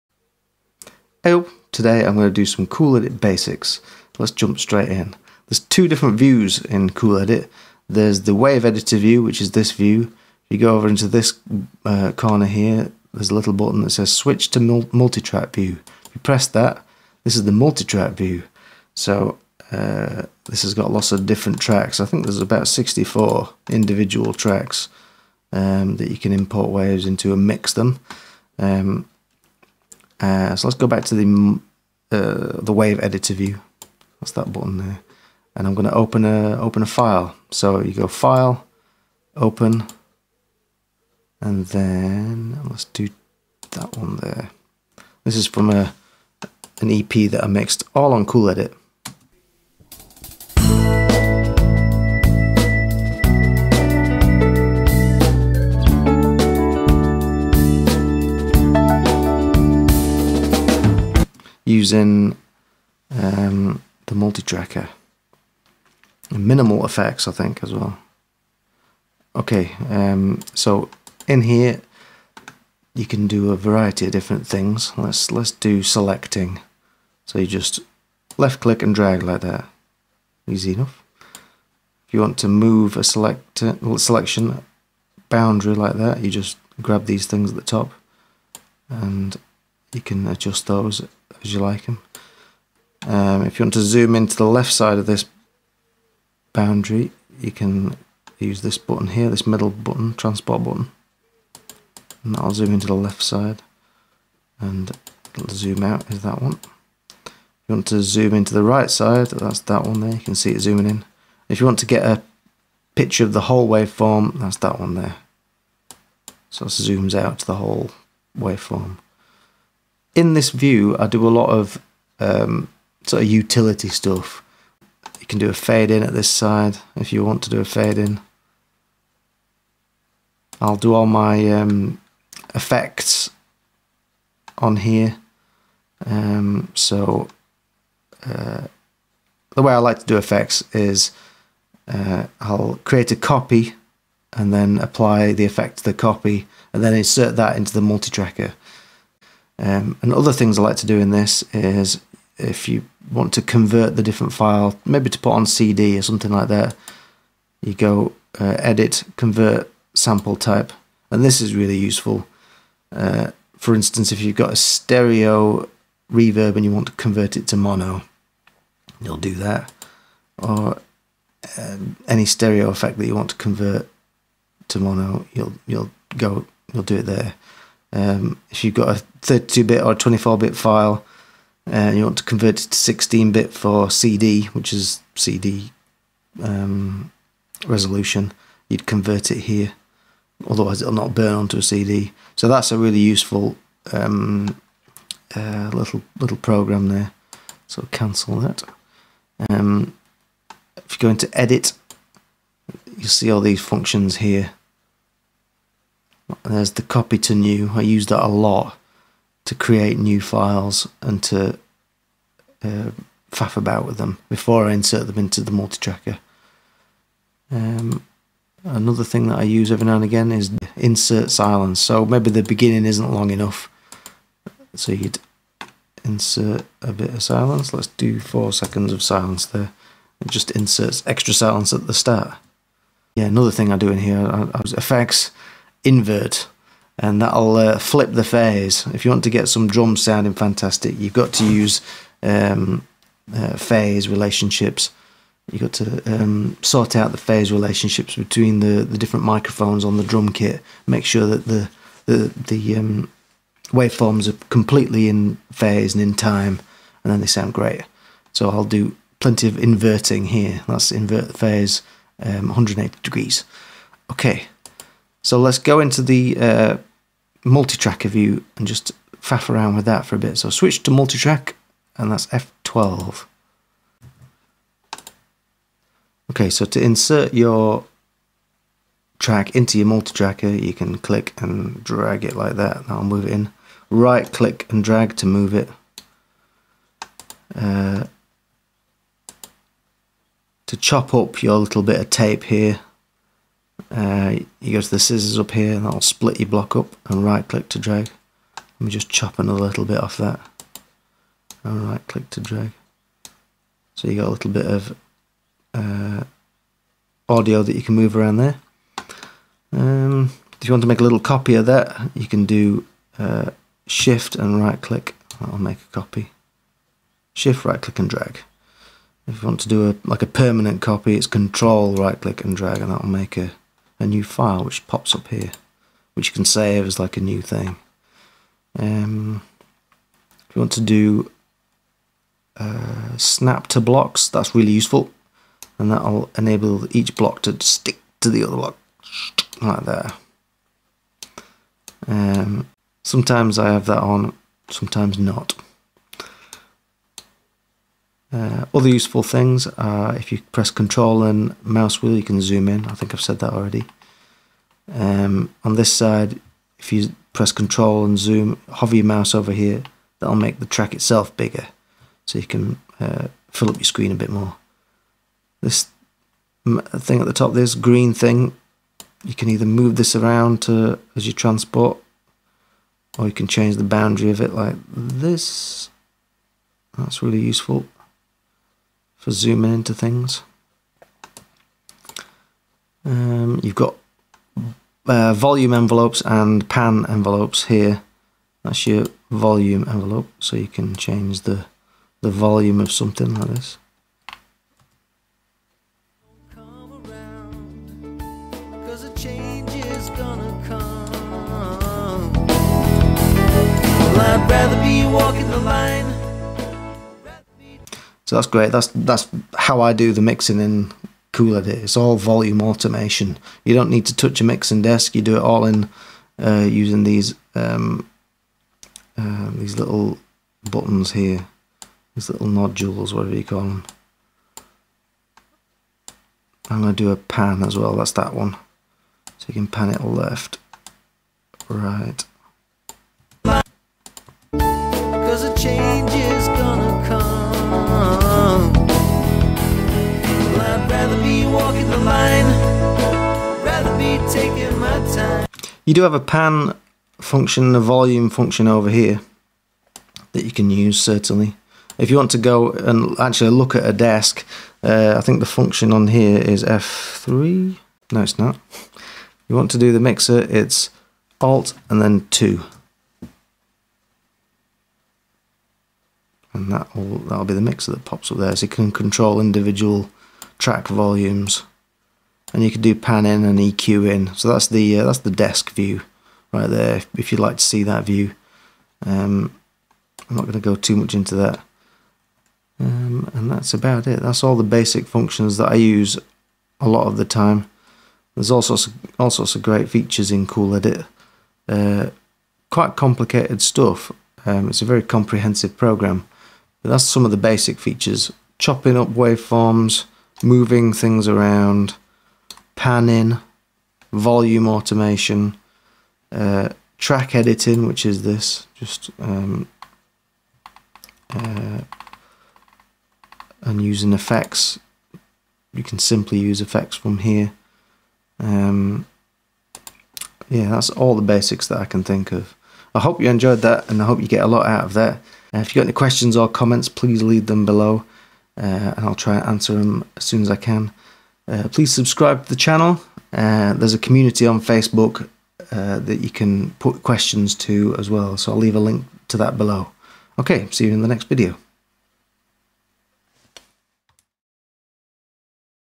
Cool Edit Pro Basics 2020
In this video, I run through the basic functions of Cool Edit 2.1pro which will allow you to import your wav files and start mixing!